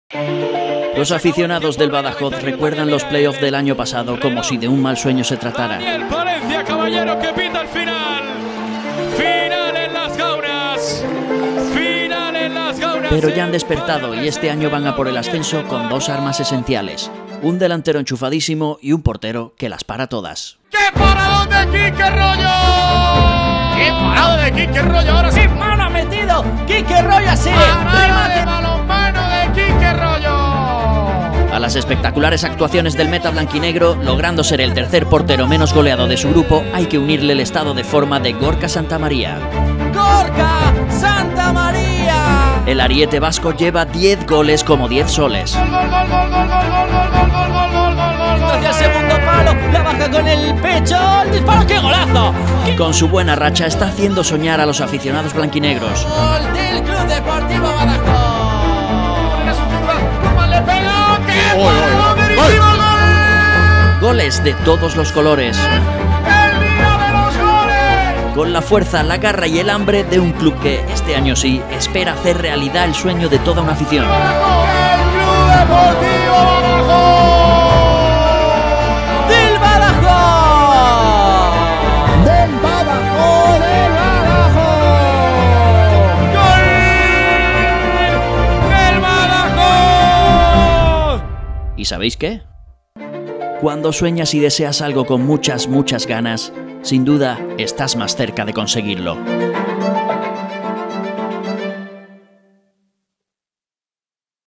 La Lupa 🔎 - Playoffs 19/20 - CD Badajoz - En busca de un sueño
Ponemos La Lupa 🔎 en un CD Badajoz con grandes aspiraciones. El club extremeño no ceja en su empeño y luchará por ascender a Segunda en los Playoffs 19/20 para alcanzar su sueño 🤔💭